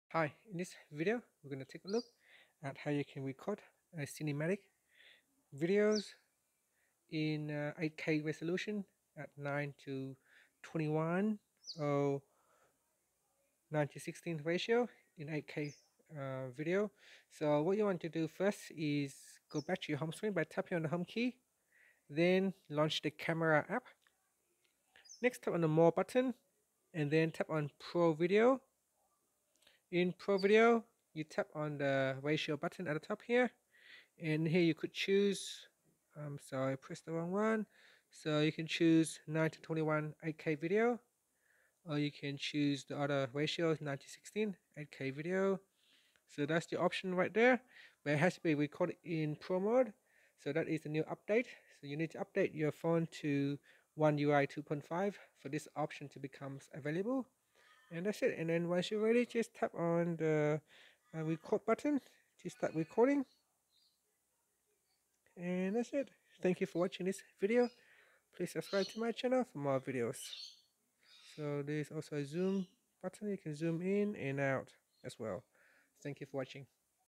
Galaxy S20/S20+: How to Record Cinematic Video In 8K Resolution 9:21
Learn how you can record cinematic video in 8K resolution 9:21 ratio on Galaxy S20 / S20 Plus / S20 Ultra.

Android 10.